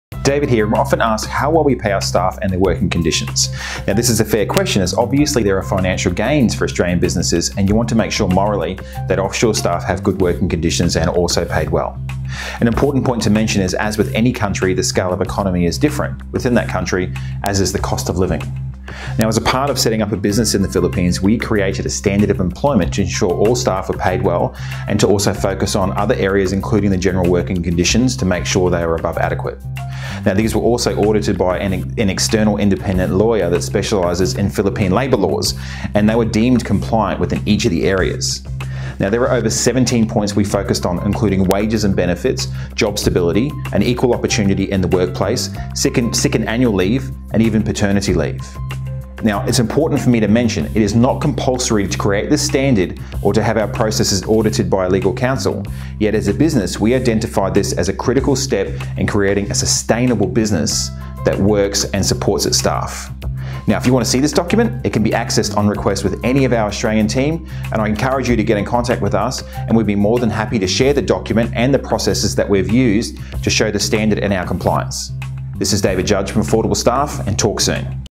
How Well Are Your Staff Treated And Paid?
It's a fair question - are Affordable Staff team members treated and paid well? In this short video, David explains how we have a Standard of Employment that ensure our staff receive above adequate pay and working conditions.